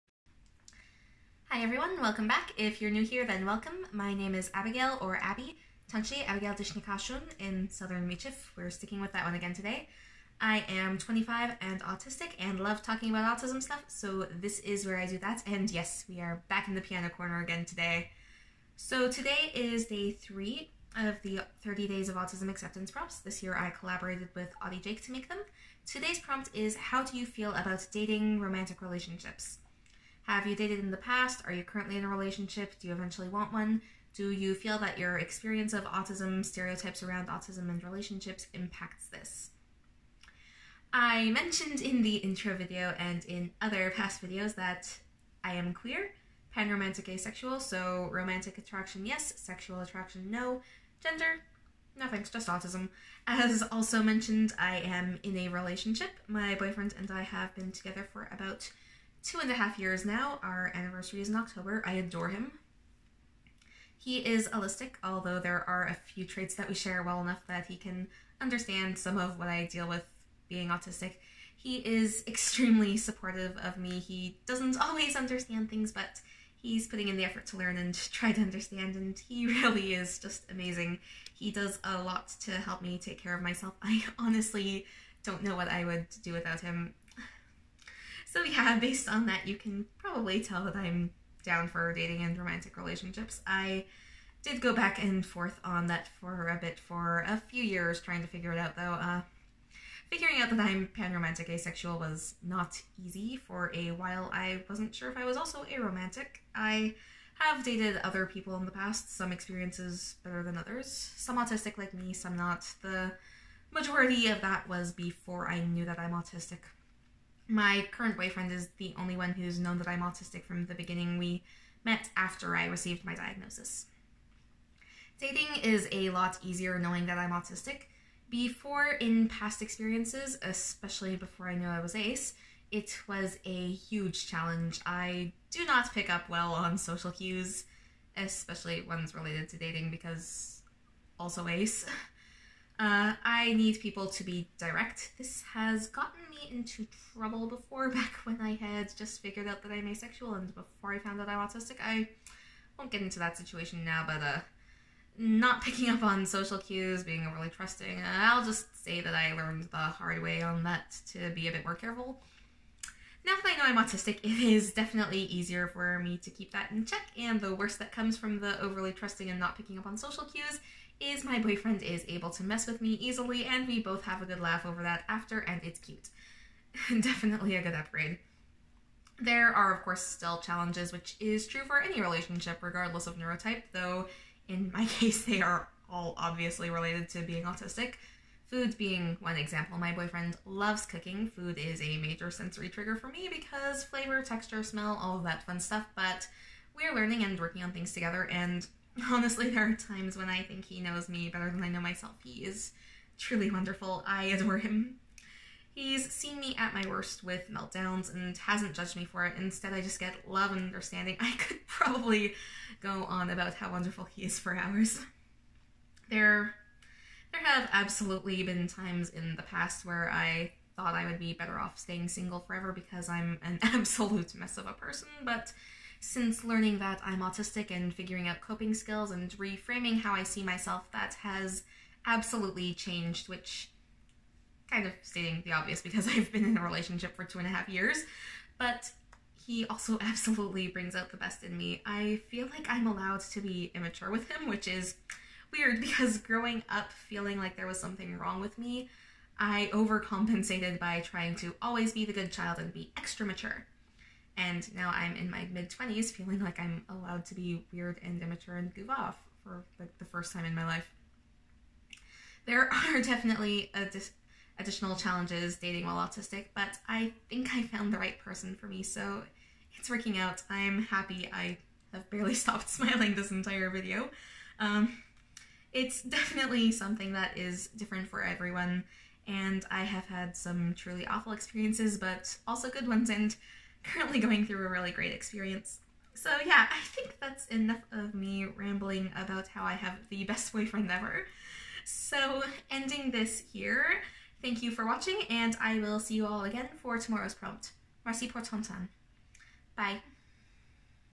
April 3: Dating and Relationships
Oops I gush about my amazing boyfriend for 5 minutes